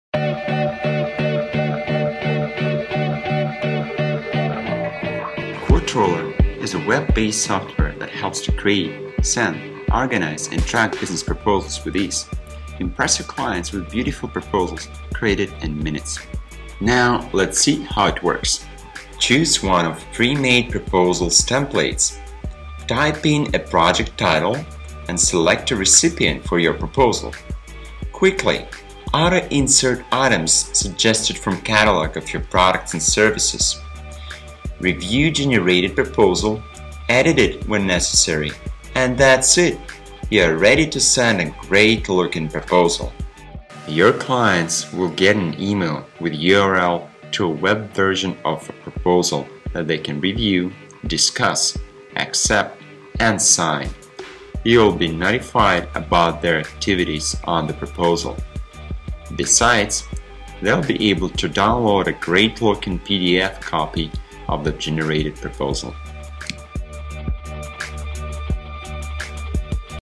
Quote Roller - create a proposals in 15 minutes
Quote Roller is a web-based proposal software that helps to create, send, track and legally electronically sign business proposals with ease. Impress your clients with beautiful proposals created in minutes.
Super easy to use, Quote Roller saves you time and helps you to close deals faster. Using Quote Roller you will find that routine proposals creation is actually a fun and enjoyable process.
There is a number of reasons why you should choose Quote Roller:
- Clients will be able to instantly accept proposals and discuss them in case of any questions.
- Organize team work on proposals creation
- Create a catalog of products and services that you can easily auto-insert in proposal body
- Create a library of templates to be used for new proposals
... any many other features that are worth checking out...